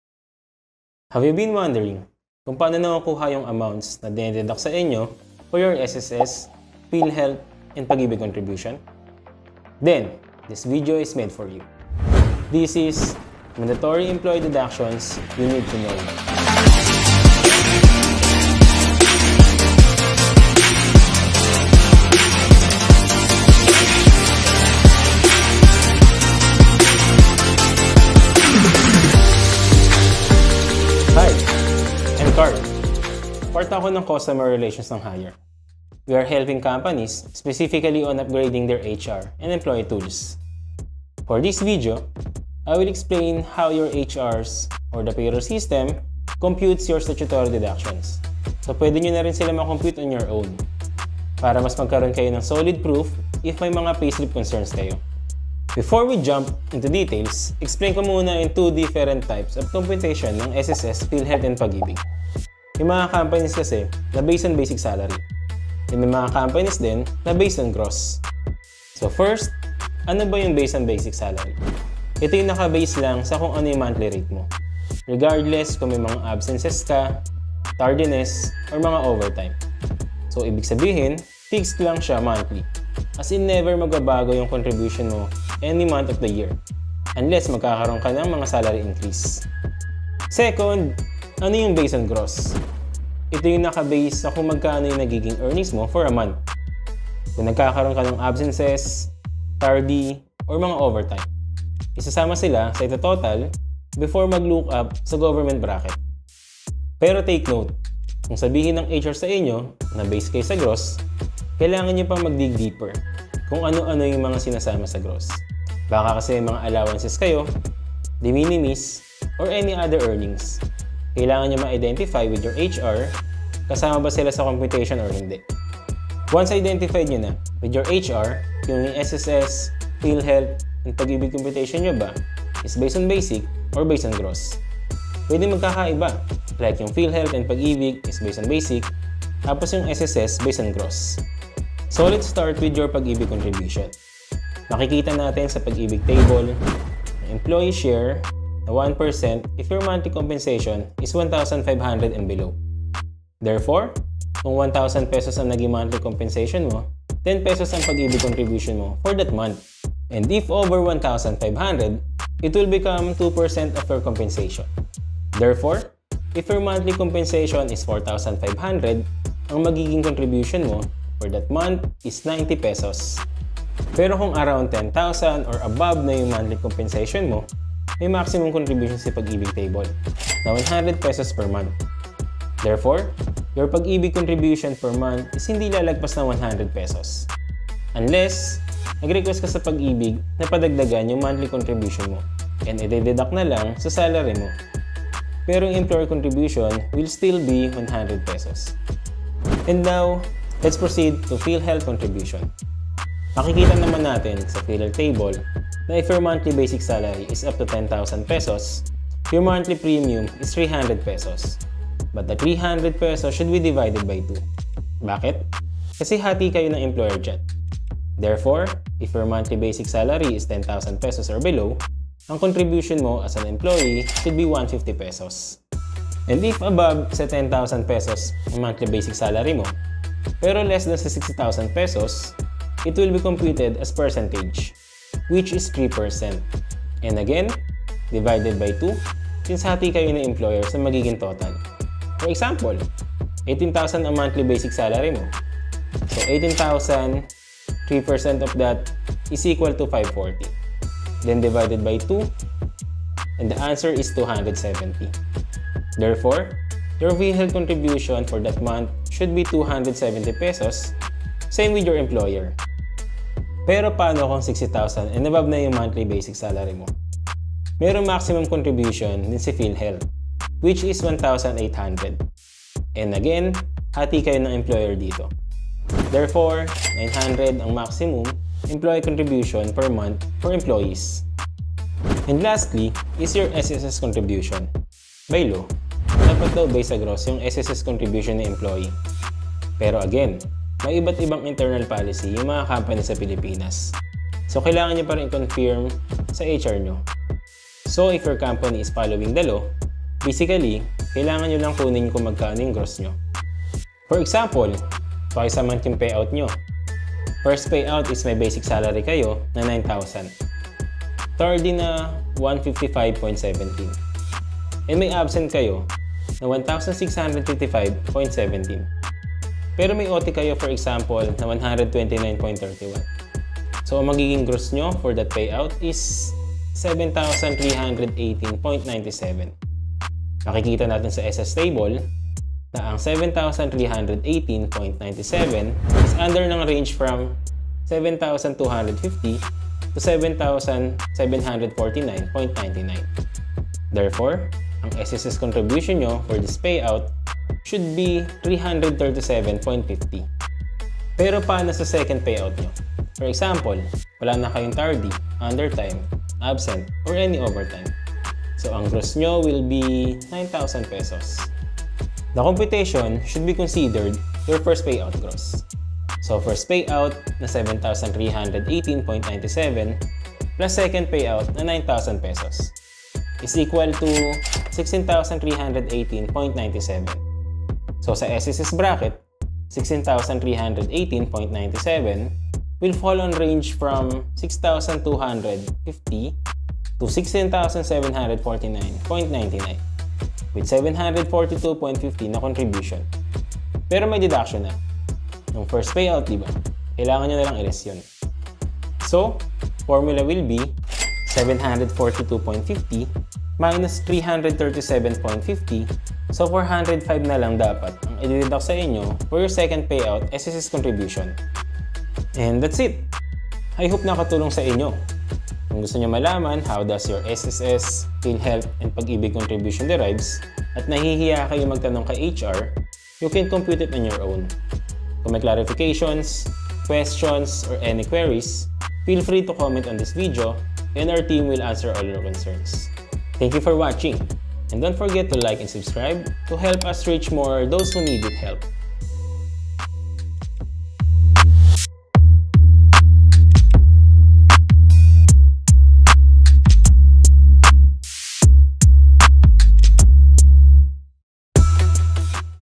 Vlog#4: Mandatory Employee Benefits you need to know
No matter what industry you’re in, you are subject to government deductions - SSS, Philhealth, and Pag-IBIG.

Deductions may either be mandatory or voluntary. The latter applies for self-employed while the former are for those who are permanently employed in the private sector.  Know how your HR or payroll systems compute your statutory deductions.

Chapters
0:00- Intro
1:11- What is based on Basic Salary?
1:33- What is based on Gross?
2:25- Computation for Pag-IBIG Contribution
3:21- Computation for Philhealth Contribution
4:44- Computation for SSS Contribution